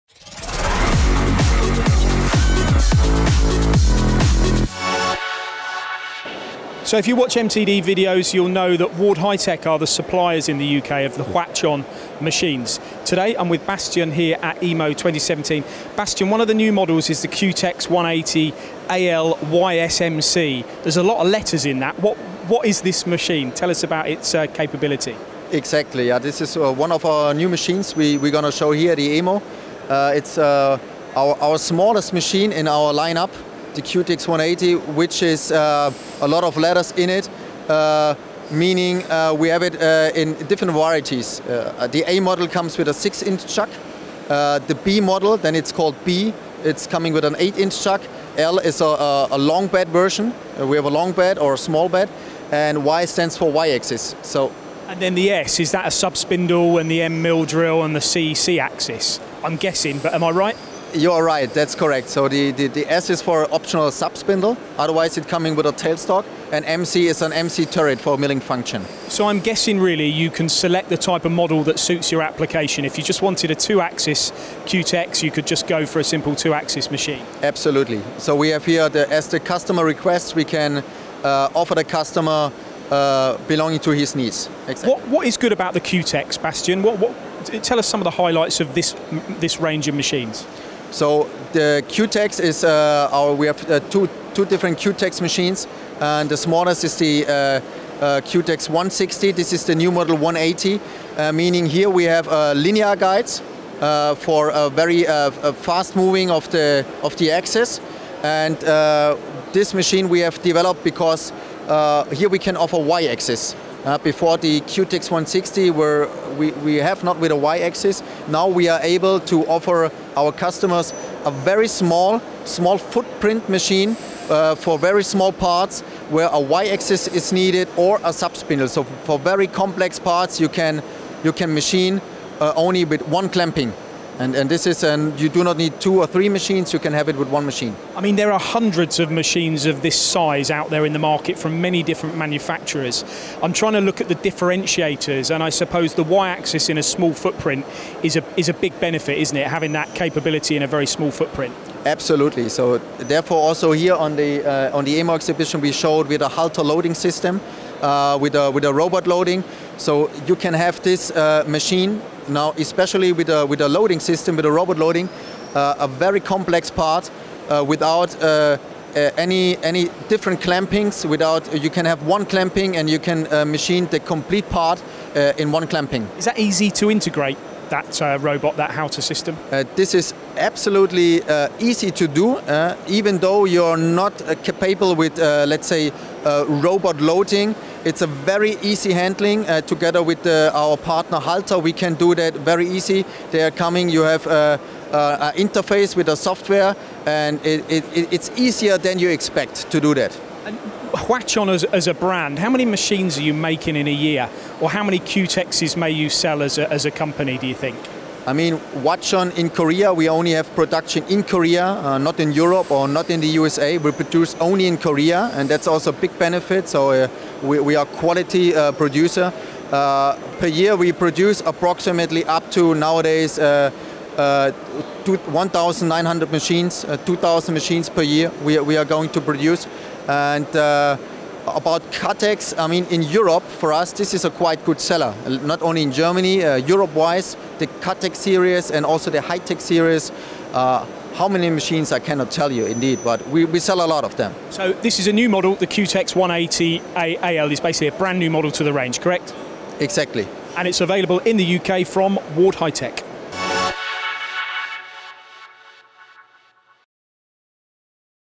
Hwacheon Cutex turning centre is the smallest in the range
Paul catches up with Hwacheon at EMO 2017. The Cutex turning centre is the smallest turning centre available in the Hwacheon range. With various options from automation to Y axis and sub spindle, this range of machines is likely to feature heavily in Ward Hi Tech's line up over the coming years.